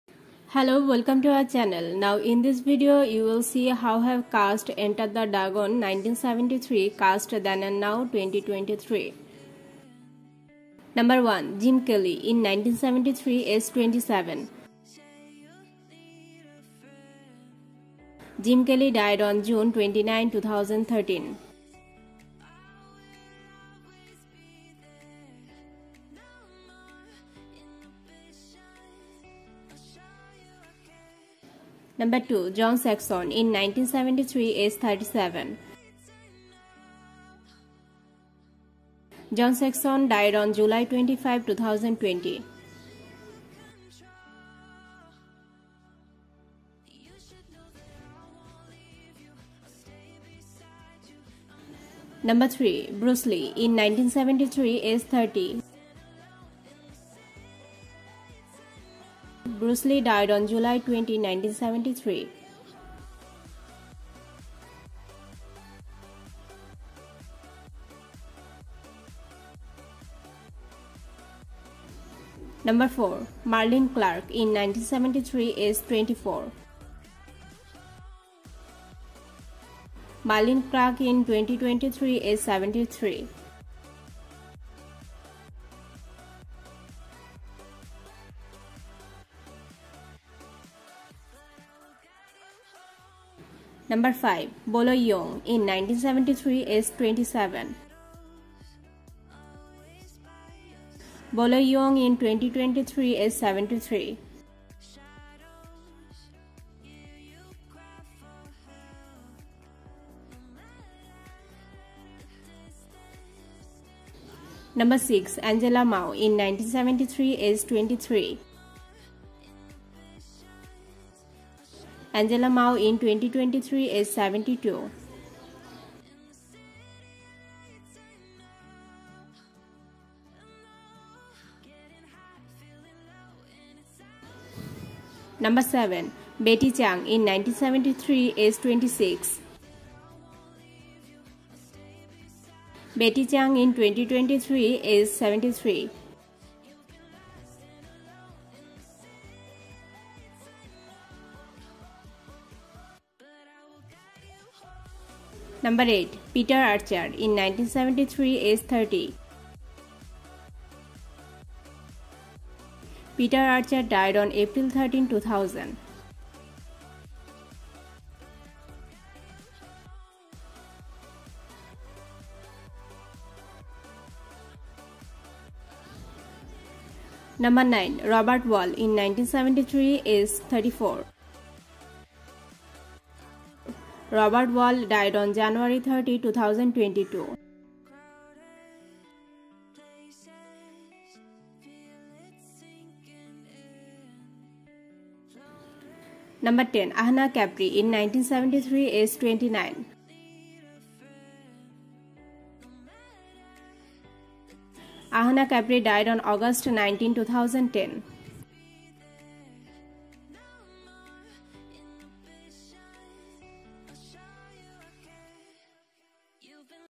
Enter the Dragon 1973 Cast Then And Now 2023 How They Changed
Enter the Dragon 1973 Main Cast
Jim Kelly
John Saxon
Bruce Lee
Marlene Clark
Bolo Yeung
Angela Mao
Betty Chung
Peter Archer
Robert Wall
Ahna Capri